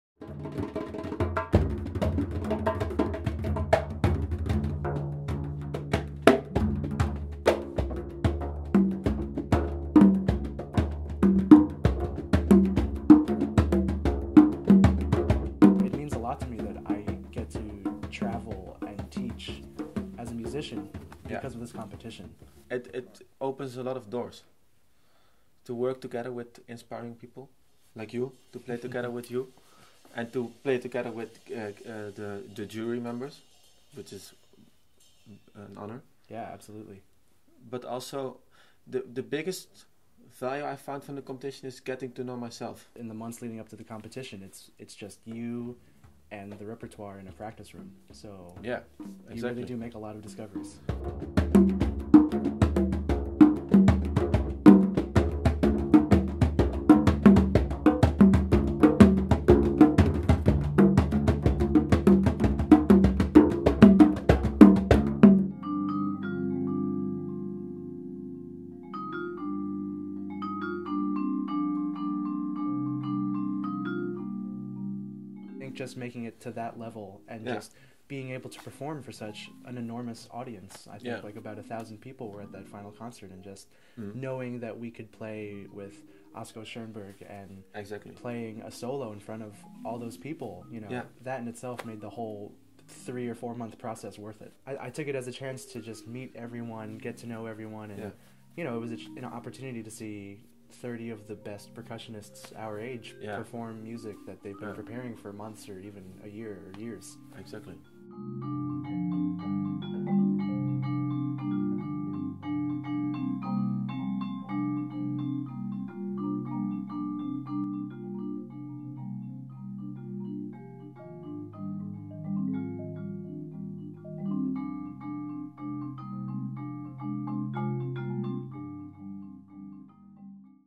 How to win the TROMP competition...
As we’ve just sent out the scores of the new mandatory pieces by Glenn Kotche and Robin de Raaff to our participants, 30 of the most talented percussionists around the world are preparing for the 2016 competition in November. Our 1st and 2nd prize winners of 2014 share what it meant for them to participate in and win the TROMP competition.

Both Dominique Vleeshouwers and Brandon Ilaw will perform at the 2016 TROMP festival in various line-ups.

TROMP Percussion Eindhoven
17/27 November 2016